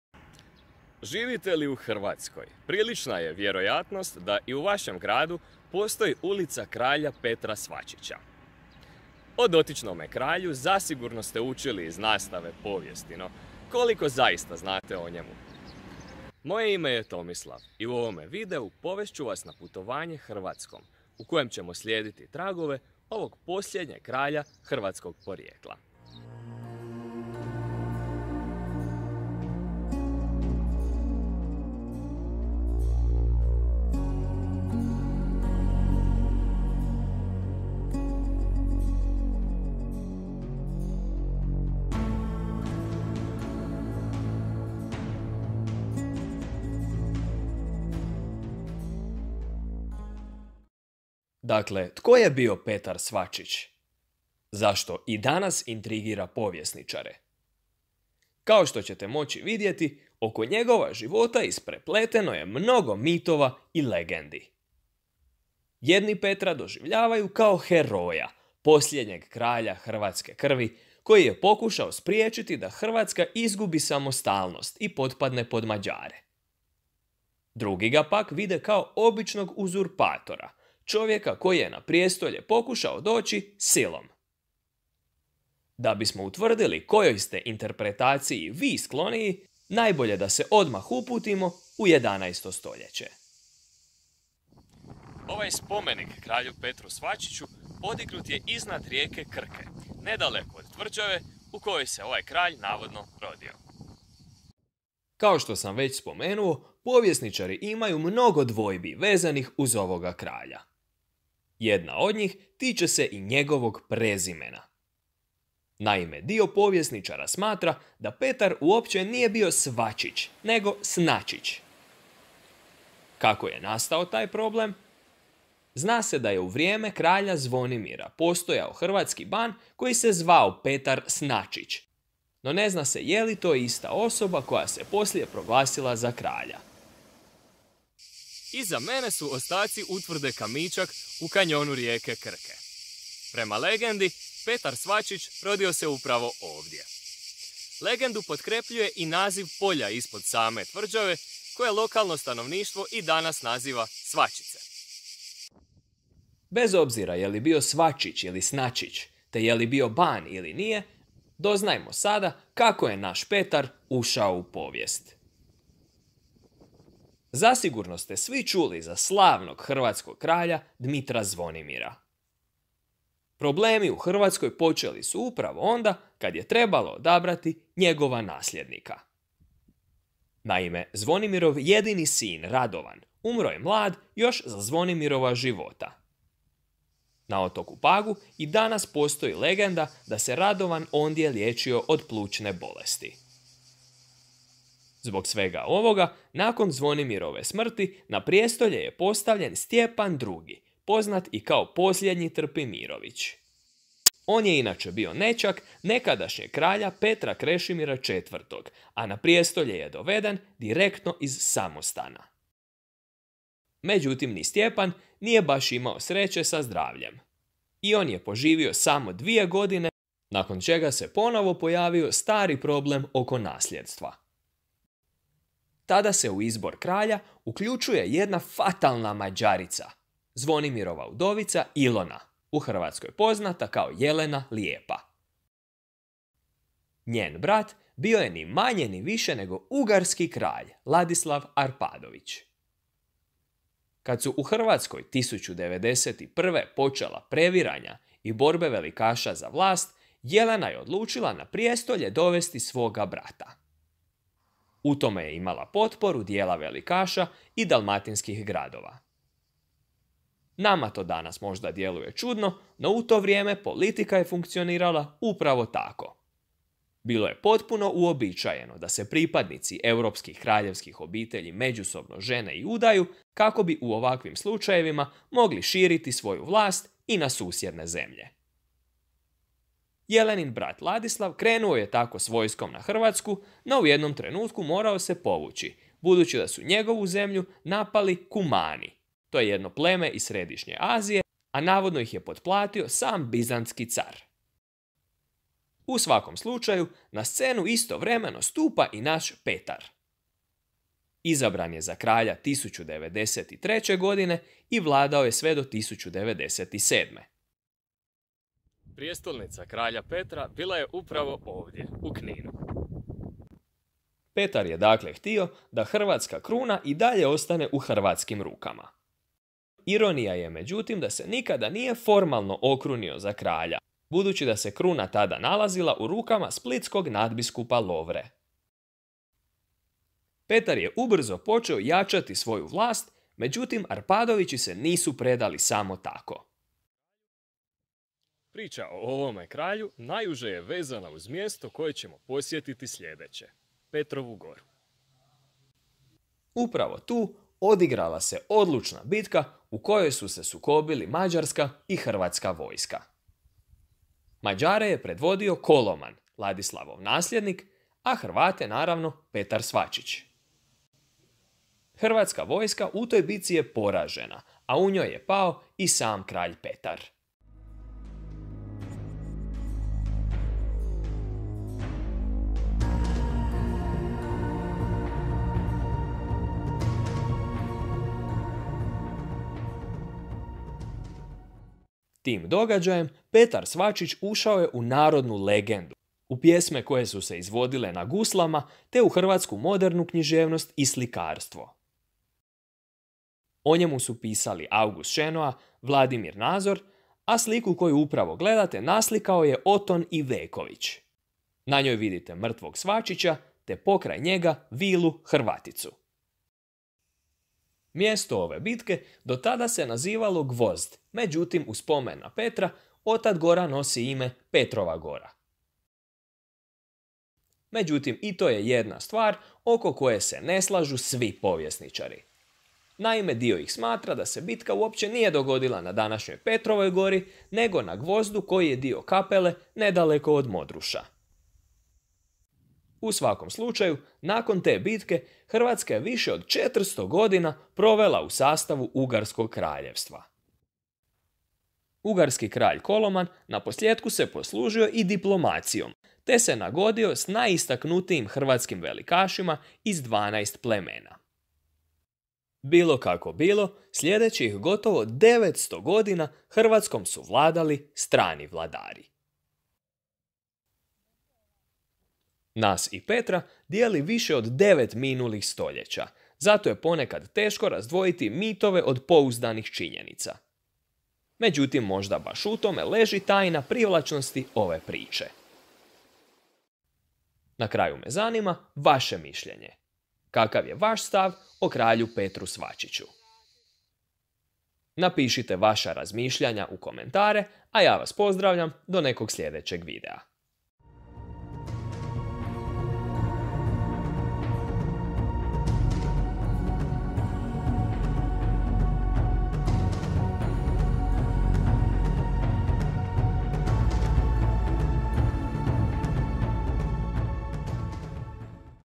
Petar Svačić - Heroj ili uzurpator?
U ovome videu doznajte tko je bio Petar Svačić, u mnogome mitska ličnost, poslije kojega su Hrvatskom gotovo 900 godina vladali strani vladari.

Fotografije korištene u videu:
- Pixabay
- Wikimedia Commons (javna domena), osim:
- Szent Lászlo spomenik, autor: Globetrotter19, licenca 3.0
- Szent Lászlo na oltaru, autor: Montecuccoli07, licenca 4.0
- Dmitar Zvonimir, spomenik u Kninu, autorica: Roberta F., licenca 3.0
- Vladimir Nazor bista, autor Slavko Stepanić, licenca 3.0